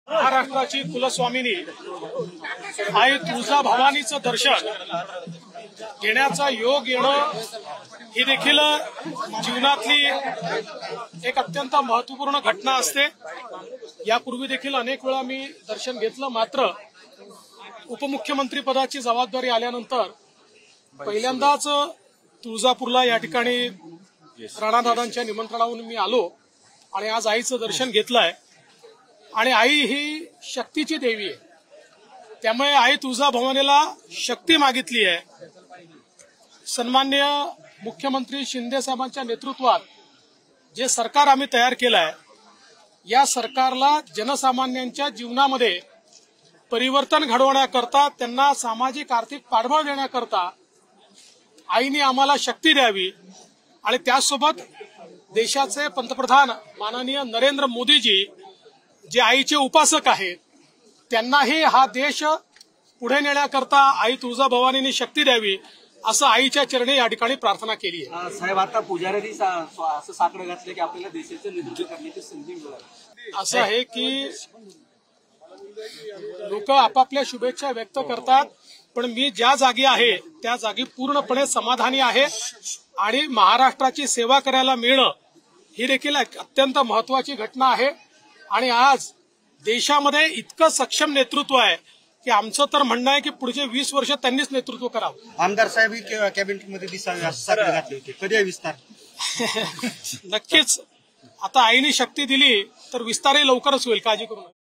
तुळजाभवानी आईचा दर्शन म्हणजे जीवनातील अत्यंत महत्वपूर्ण क्षण! | तुळजापूर | DCM Devendra Fadnavis
तुळजाभवानी आईचा दर्शन म्हणजे जीवनातील अत्यंत महत्वपूर्ण क्षण! | तुळजापूर | DCM Devendra Fadnavis | 16th जून 2023